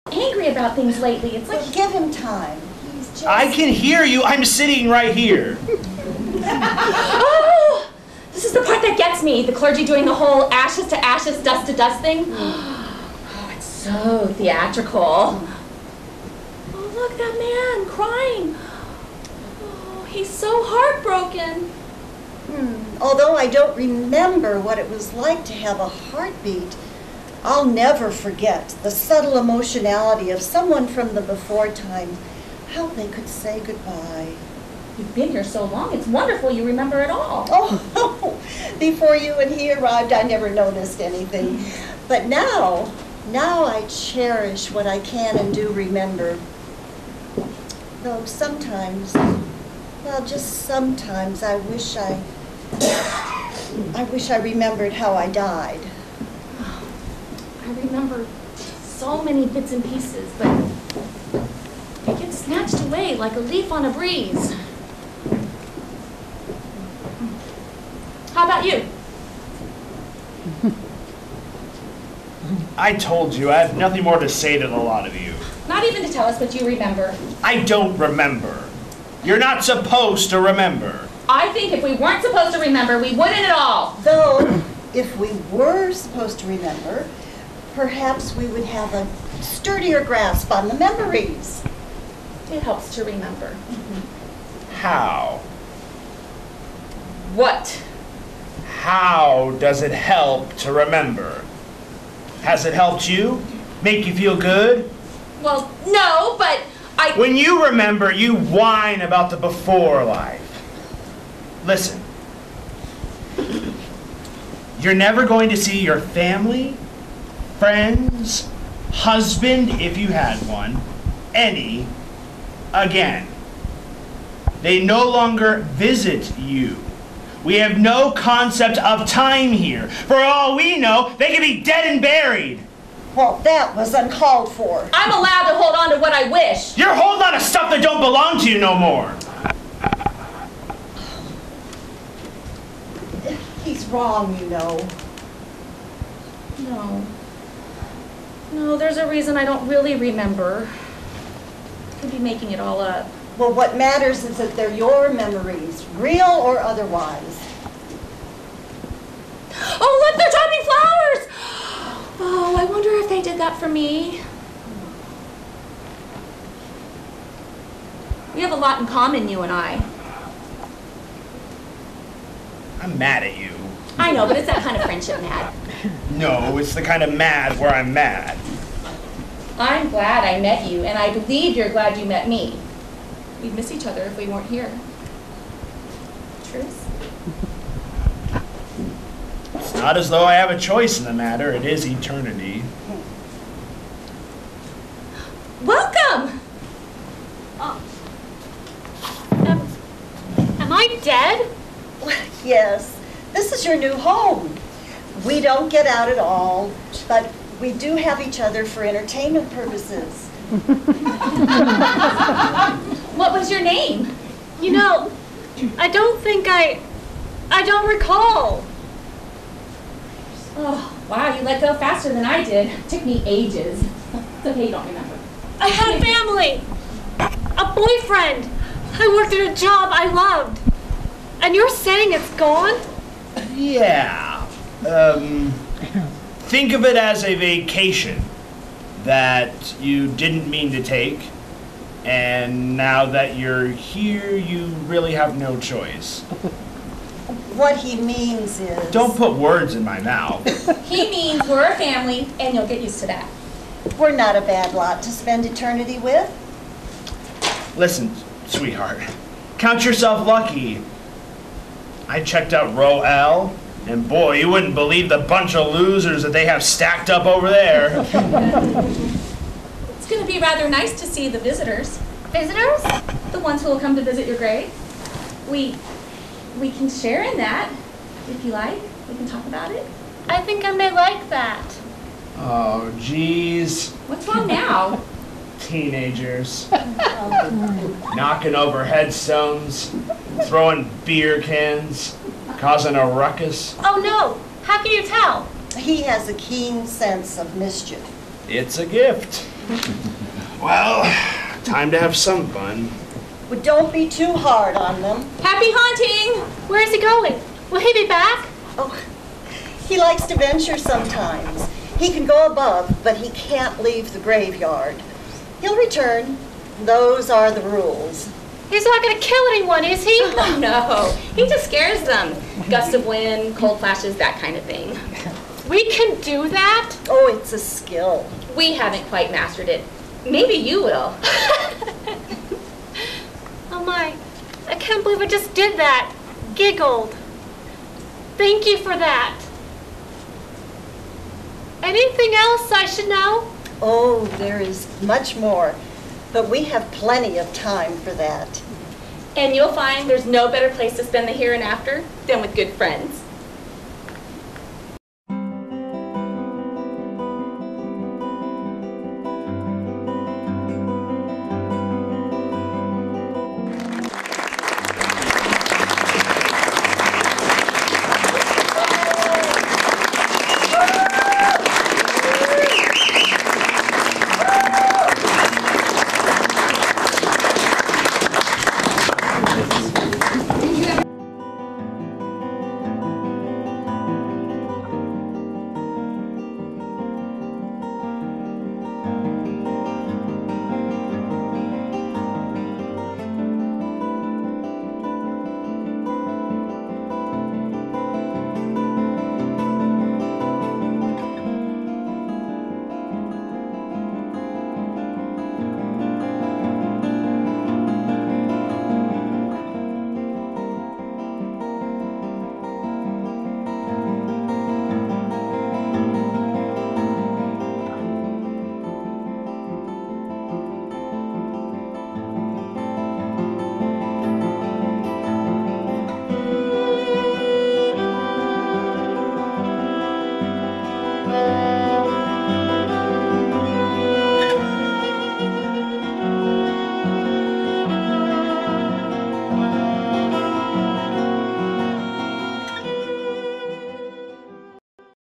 Minuet at the Dog Park and Spirit Part 2 - Written and Directed by Larissa Brewington
Written and Directed by Larissa Brewington -Herberger Festival of the Arts 10/5/2013 -

MINUET AT THE DOG PARK – Featuring Bob Barr and Marlene Woods

Plot: Strangers meeting at a dog park, find they have a lot in common.

SPIRIT  – Featuring Michelle Y. Allen, Madelyn Rusthoven, Tony Latham and Anna Sahlstrom
Plot: Life and friendship can be found in the after life.